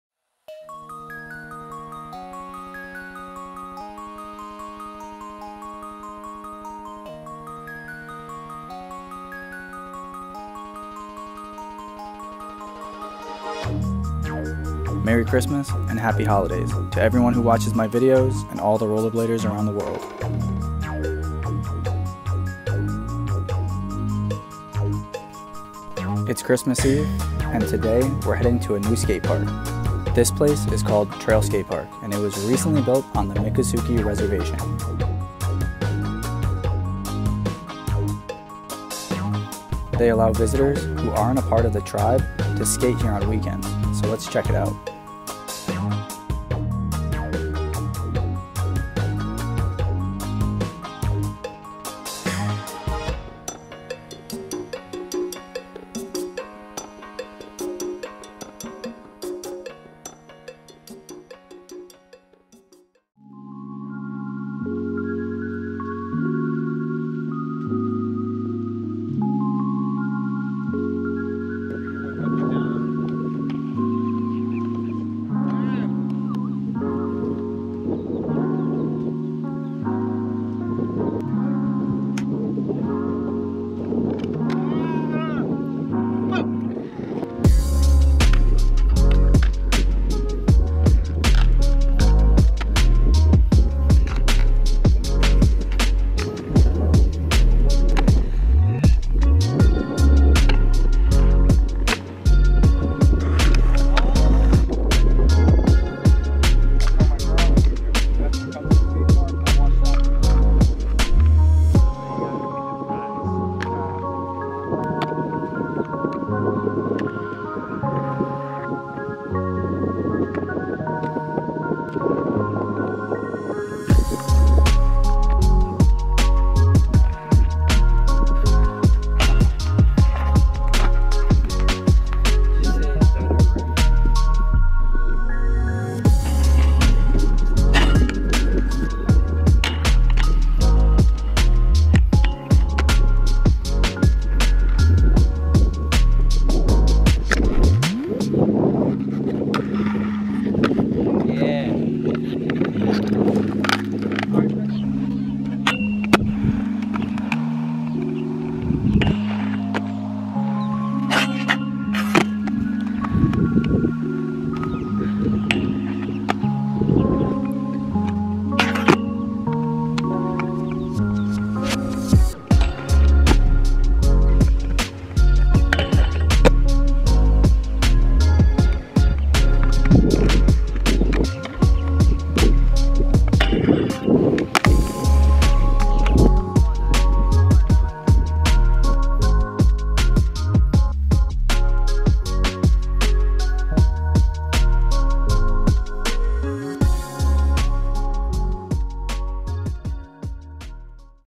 New Skatepark Built in the Middle of the Everglades & It's Amazing | Miccosukee Trail Skate Park
First session on Christmas eve at the newly built Miccosukee Trail Skatepark in the South Florida everglades.

You see skate sessions, vlogs, media coverage, competition coverage and more!